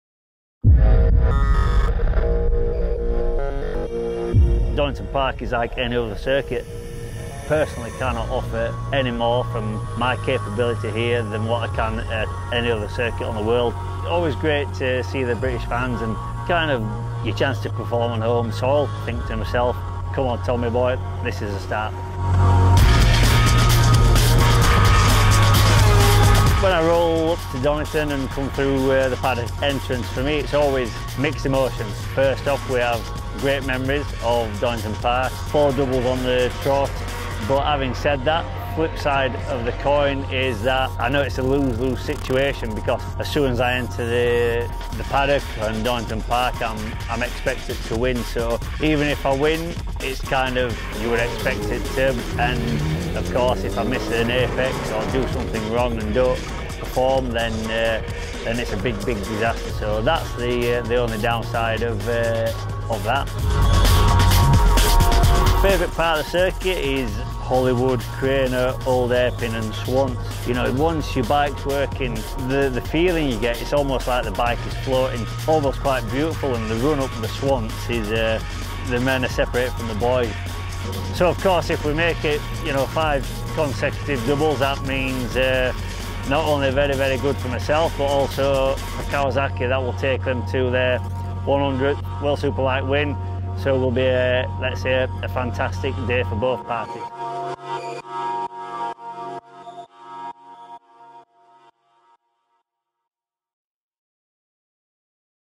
Donington Dreams Part 1
Tom Sykes has a "special relationship" with Donington Park having won eight races in a row here in World SBK. Watch as he tells us his thoughts about the track, his feelings about the pressure of success and the imminent prospect of 100 Kawasaki wins in World SBK.